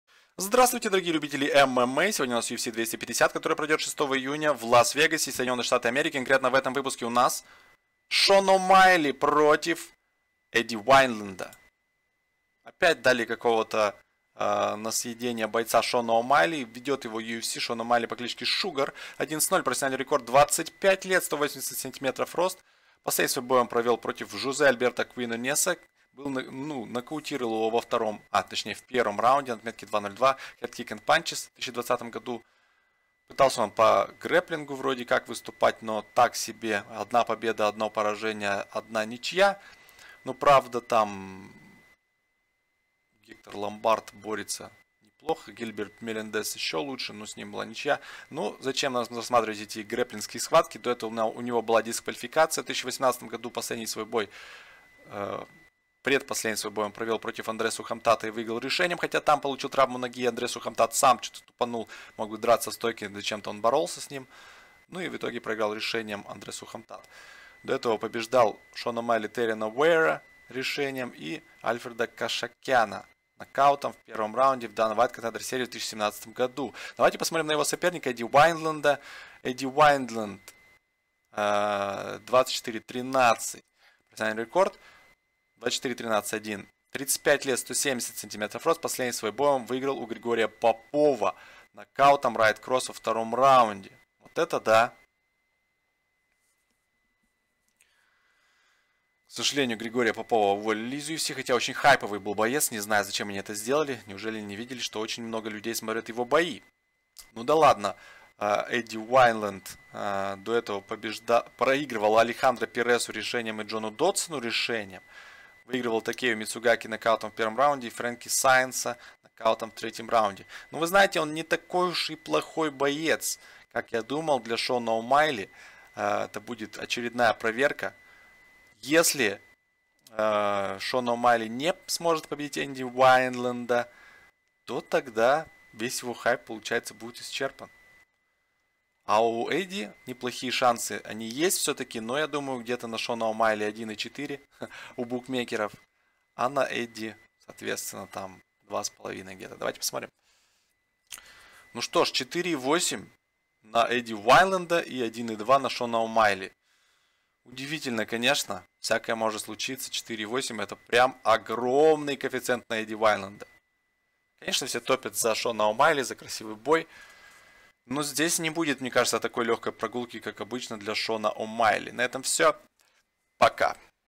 UFC 250: Шон О Майли vs. Эдди Уайнлэнд
Подробный обзор предстоящих поединков и онлайн прогнозы